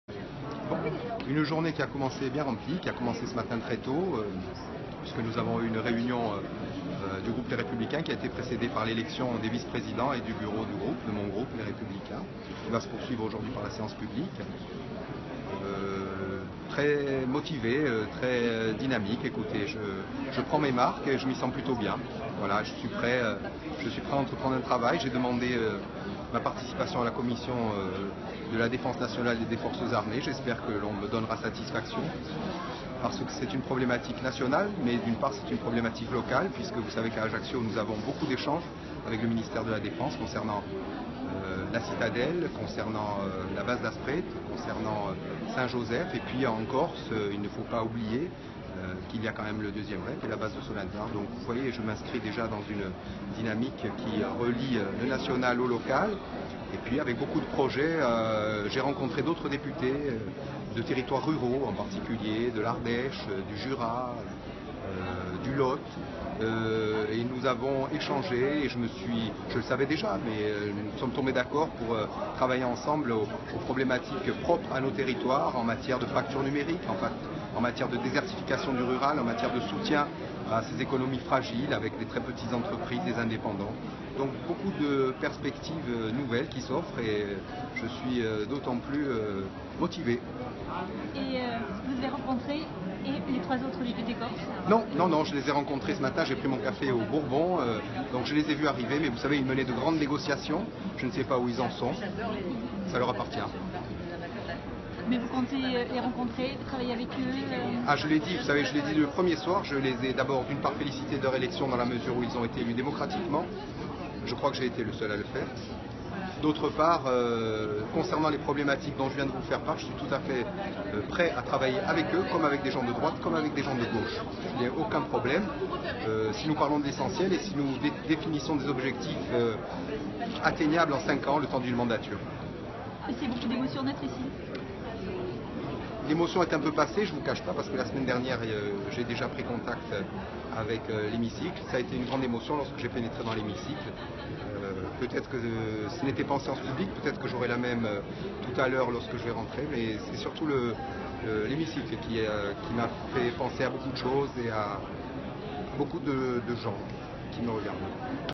Corse : L'émotion de Jean-Jacques Ferrara
Jean-Jacques Ferrara député de la 1ère circonscription de la Corse-du-Sud a fait son entrée à l'Assemblée nationale en même temps que ses trois autres homologues de Corse. Il livre ses impressions, non sans émotion, avant de s'installer officiellement dans l'hémicycle